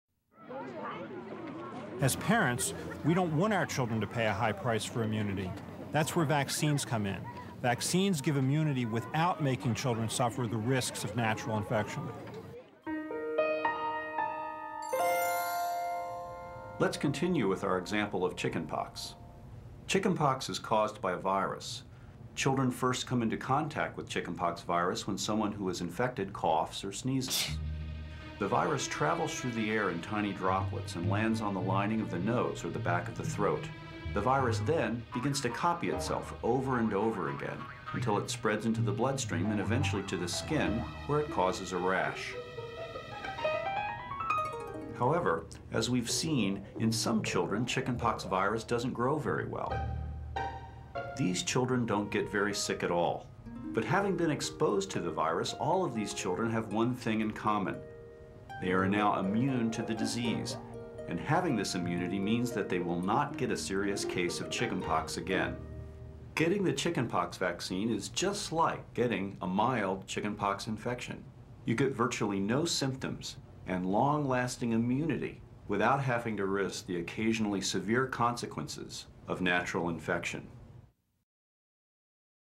What is Immunity? - Vaccines and Your Baby - The Children's Hospital of Philadelphia (3 of 14)
Paul Offit, MD, explains how children become immune to diseases through vaccination.

Vaccines and Your Baby explains the basics of vaccines. You'll learn about vaccines and how they work, the science behind vaccines, immunity, and the diseases that vaccines prevent. Parents of children who suffered vaccine-preventable diseases tell their stories.